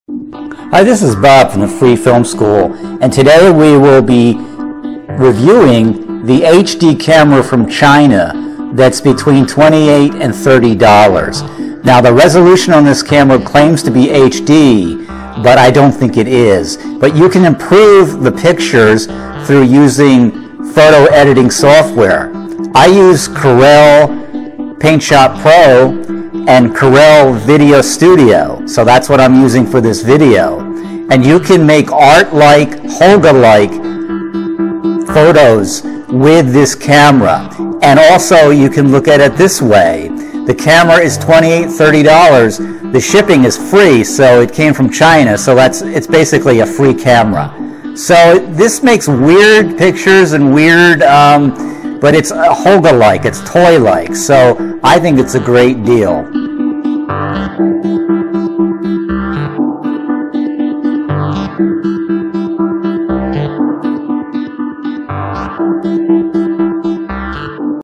30 Dollar DSLR Camera The Holga Beater
30 Dollar DSLR Camera The Holga Beater: For the price of $30 or in my case $28 you can have a Fake HD Camera from China. It is not a very good HD camera but it's makes for a Cheap Holga Like, Plastic Fantastic, Digital Toy, Art Camera, for Under or at $30. A low Res gem. Just add photo editing software such as Corel Paintshop Pro or any version, old or new,  of Adobe Photoshop, Lightroom, or arty apps you have on your phone, email the pics to yourself and edit & send them on your phone. Warhol used Polaroids!